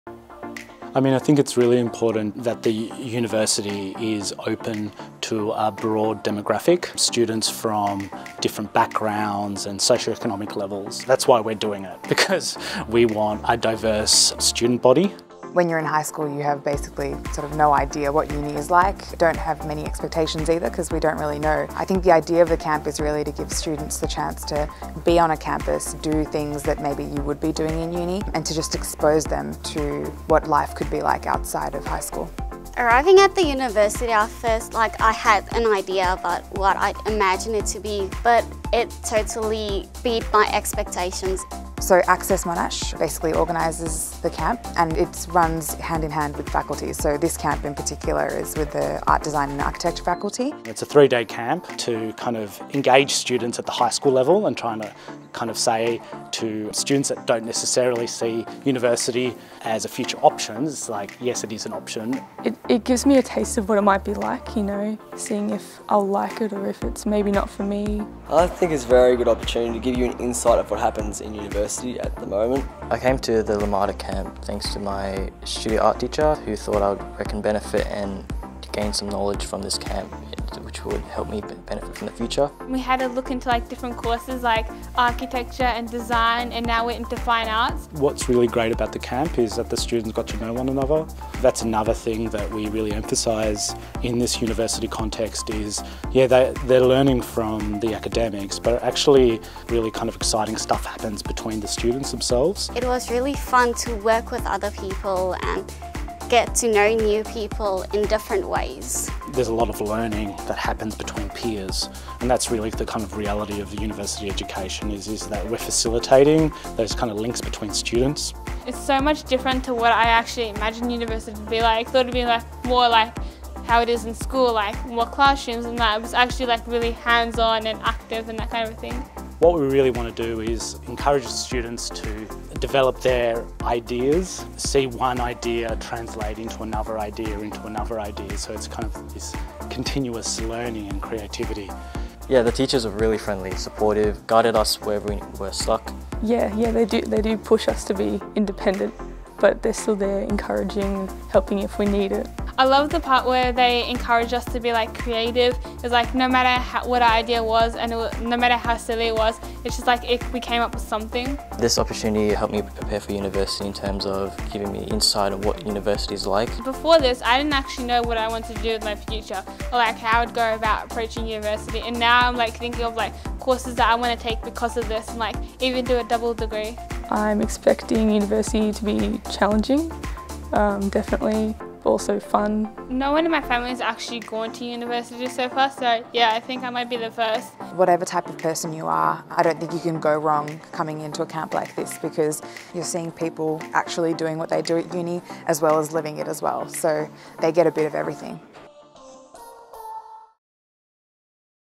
Access Monash | Learning and Making in Art Design and Architecture
A three-day experience giving students a first-hand insight into studying and working in the creative arts. It’s a chance for students to get a head start on their Art, Studio Art, Visual Communication and Product Design and Technology folios in hands-on design lab sessions. They also find out what it’s like to work in creative fields such as architecture and visual communications.